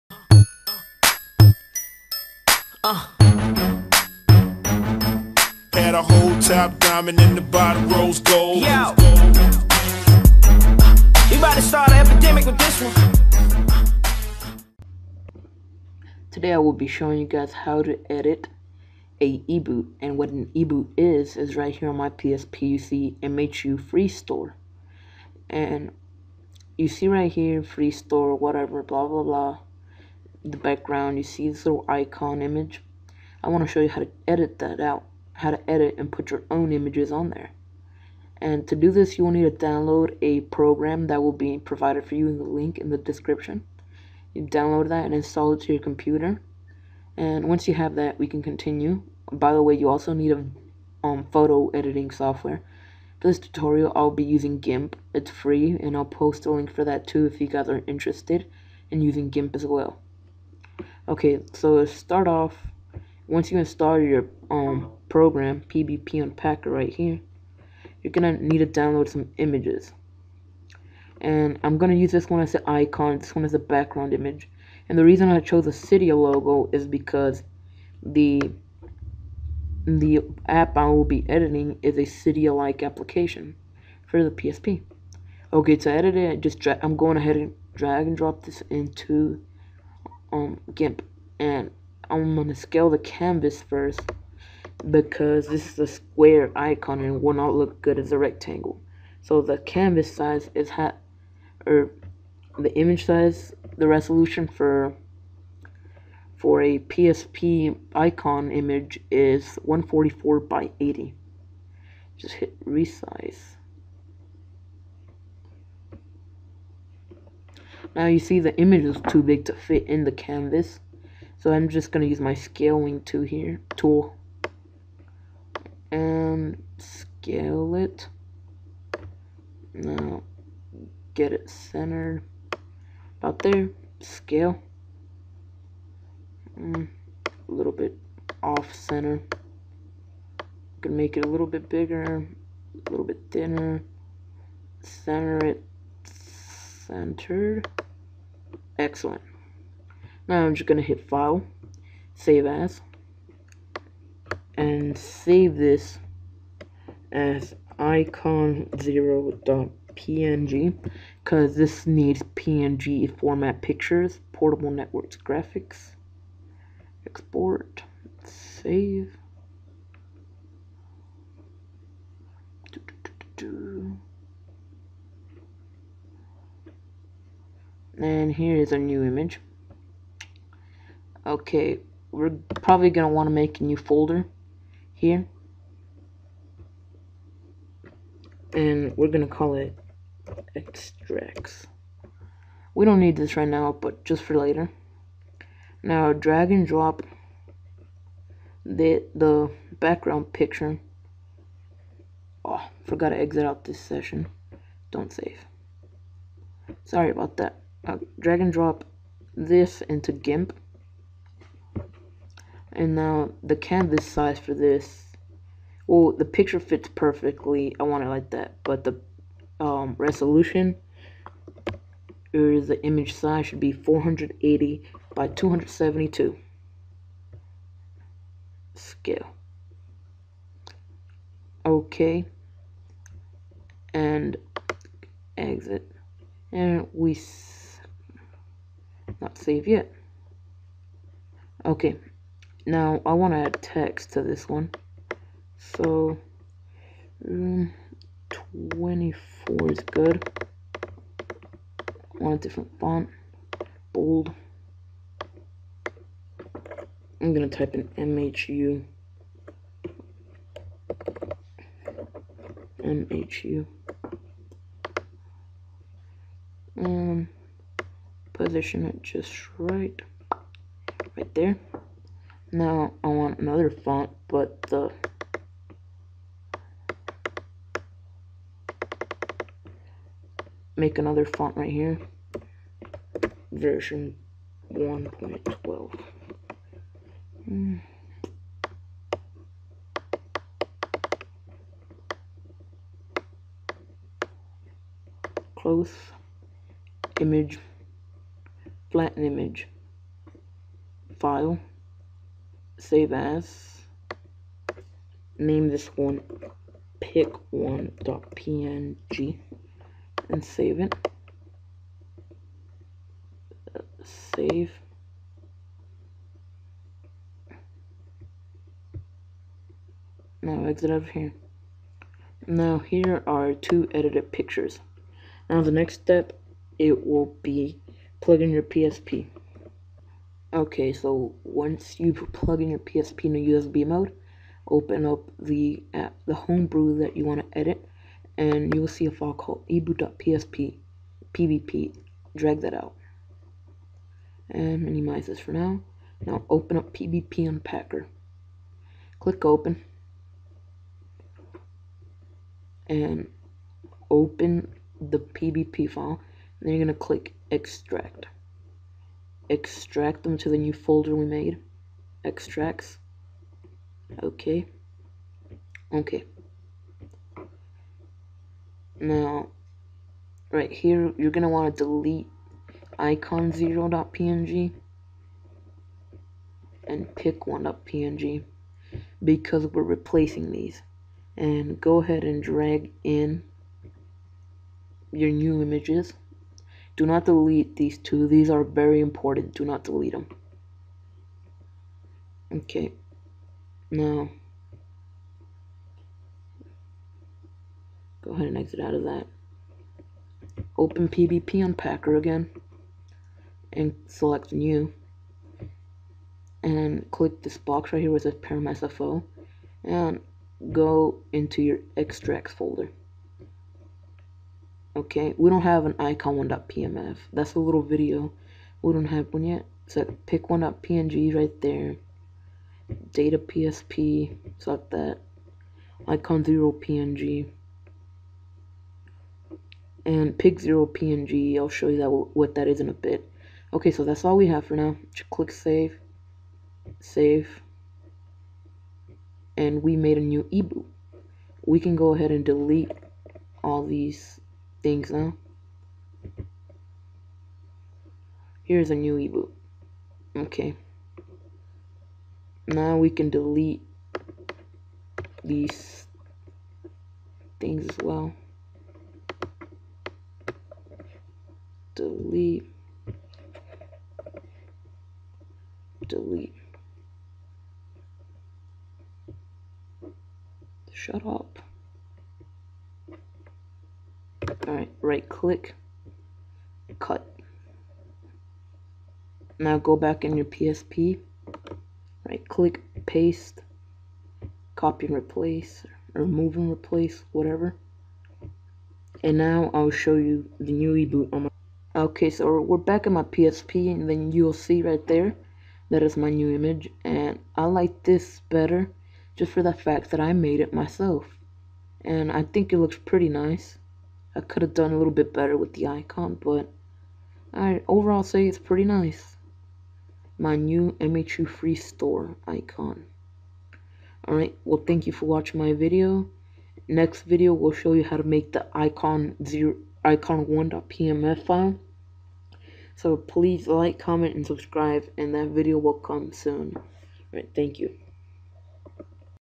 Tutorial:PSP How to customize Eboots Part 1 (Icon/Background) [HD]
Hello YouTube Its David Here and today I will be showing you my tutorial on customizing a eboot.